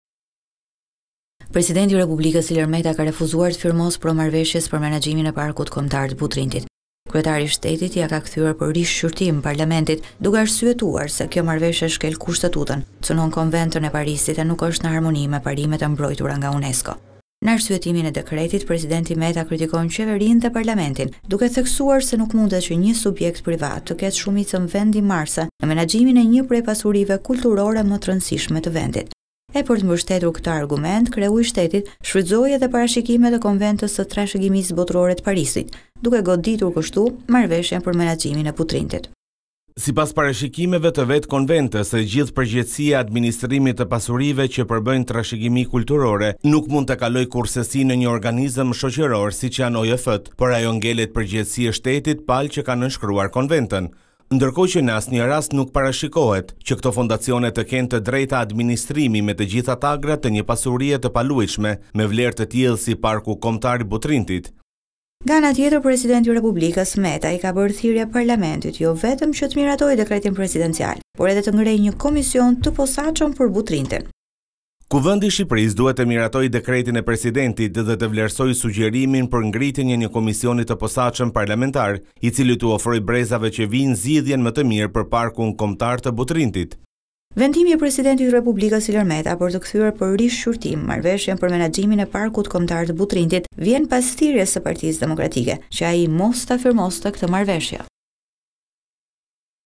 Meta kërkesë Kuvendit: Të rikthehet për shqyrtim çështja e Parkut Kombëtar të Butrintit -
Presidenti i vendit, Ilir Meta i është drejtuar kuvendit me një kërkesë për ngritjen e një komisioni të posaçëm për Parkun Kombëtar të Butrintit. Meta ka kërkuar rikthimin në Kuvend që kojo çështje të marrë kohën dhe vëmendjen e duhur përmes një procesi real dhe të thelluar diskutimi. Kërkesa e plotë:Me qëllim:- respektimin e parimeve kushtetuese dhe vlerave universale;- mbrojtjen e trashëgimisë tonë kombëtare nga shtrembërime të çdo lloj natyre;- respektimin e Konventës së Parisit dhe parimeve të përcaktuara nga UNESCO;- për të garantuar që rendi juridik i normave të legjislacionit shqiptar, është i njëjtë dhe i barabartë për të gjithë dhe askush nuk mund të ketë një trajtim të parapëlqyer nga qeveria nëpërmjet Kuvendit;- për të mos lejuar asnjë mundësi që pasuria e shqiptarëve të keqadministrohet dhe pjesët më të rëndësishme të saj që faktojnë gjurmët reale të identitetit ilir dhe shtetit shqiptar të humbasin;- në respekt të Rezolutës së Parlamentit Evropian për Shqipërinë të datës 19 maj 2022 dhe i bindur dhe mbështetës për gjithë sa parashtrohet në këtë Rezolutë;- me qëllimin që pasuria dhe financat e shtetit që krijohen nga një pasuri kombëtare e patjetërsueshme, të administrohen në funksion të kësaj çështje në mënyrën më të mirë të mundshme;- duke respektuar njëkohësisht dhe vullnetin dhe angazhimin prej vitesh të Fondacionit Shqiptaro- Amerikan për Zhvillim (AADF);- në respekt të çdo eksperti dhe qytetari dhe anëtari të shoqërisë civile të Republikës së Shqipërisë, i cili përmes opinioneve, vlerësimeve dhe angazhimeve kërkon mbrojtjen dhe promovimin e vlerave tona kombëtare dhe pasurisë sonë të përbashkët;mbështetur në nenin 85 pika 1, 86 pika 1, 92/a, të Kushtetutës, si Presidenti i Republikës, në cilësinë e Kreut të Shtetit dhe si përfaqësues i unitetit të popullit, kam vendosur që ky ligj duhet të rikthehet edhe një herë në Kuvend, me qëllim që kjo çështje, të marre kohën dhe vëmendjen e duhur, për t`u vlerësuar përsëri nga Kuvendi, përmes një procesi real dhe të thelluar diskutimi.Lidhur me këtë çështje kaq të rëndësishme, si President i Republikës, vlerësoj se në mbështetje të Kushtetutës së Republikës së Shqipërisë, si dhe nenit 24 të Rregullores së Kuvendit , kjo çështje, hyn pikërisht në llojin e atyre çështjeve të një rëndësie ekstremisht të veçantë, e cila kërkon kujdes të lartë nga organet shtetërore dhe për këtë Kuvendi i Shqipërisë, duhet të miratojë dekretin e Presidentit, dhe të vlerësojë sugjerimin për ngritjen e një Komisioni të Posaçëm Parlamentar, i cili përmes një procesi të gjerë dhe solid konsultimi e diskutimi, të mund të prodhojë produktin më të mirë, e përmes një dakordësie të gjerë t`u ofrojë brezave që vijnë zgjidhjen më të mirë për Parkun Kombëtar të Butrintit dhe gjithë pasuritë të çdo lloji pjesë e tij.Në këtë mënyrë, jo vetëm do të garantojmë zgjidhje dhe zgjedhje më të mira, por do të ishim në unison dhe me kërkesat e Parlamentit Evropian i cili në datën 19 maj 2022, ka miratuar Rezolutën me përplot 73 pika, ku ndër të tjera, bën thirrje që politika shqiptare të tregojë pjekuri politike në trajtimin e çështjeve me interes publik, të rrisë demokracinë, rikujton përgjegjësinë e autoriteteve qendrore dhe lokale për të siguruar transparencë, llogaridhënie dhe gjithëpërfshirje, si dhe thekson se përmirësimi i qeverisjes, transparenca, siguria ligjore, përfshirja dhe dialogu social janë thelbësore për stimulimin e investimeve të huaja direkte dhe mbajtjen e punëtorëve të kualifikuar; bën thirrje për një përfshirje kuptimplotë të shoqërisë civile dhe palëve të tjera të interesuara në debatin mbi veprimet e politikave.Nëse shumica në Kuvendin e Shqipërisë do të zgjedhë siç dhe ka deklaruar publikisht të rrëzojë politikisht dekretin e Presidentit të Republikës, dhe të vijojë me hyrjen në fuqi të ligjit nr. 50/2022, kjo nuk do t`i shërbente aspak interesave të larta të vendit dhe kombit shqiptar, por do të përkeqësonte çdo marrëdhënie të organeve të shtetit me shoqërinë civile, ekspertët, dhe qytetarët në tërësi.